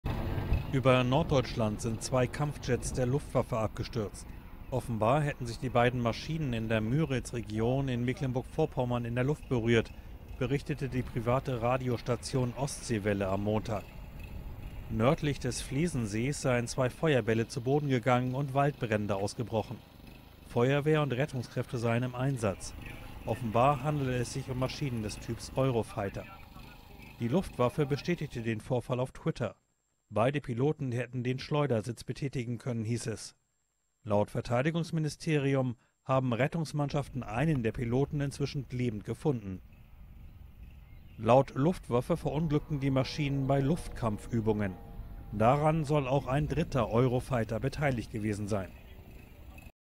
Zwei „Eurofighter“ über Norddeutschland abgestürzt
Über Norddeutschland sind zwei Kampfjets der Luftwaffe abgestürzt. Offenbar hätten sich die beiden Maschinen in der Müritzregion in Mecklenburg-Vorpommern in der Luft berührt, berichtete die private Radiostation Ostseewelle. Einer der Piloten wurde lebend aufgefunden.